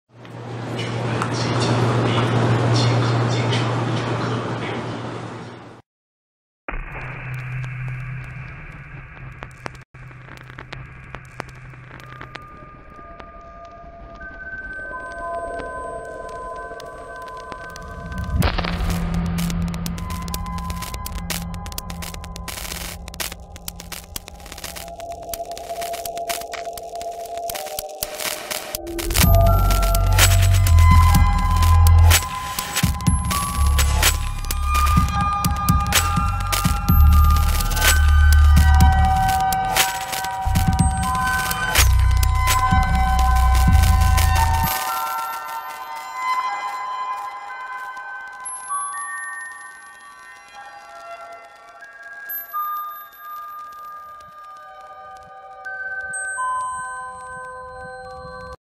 Touch
It is a piece choreographed and performed by Bitao Cheng & Yingdong Zhao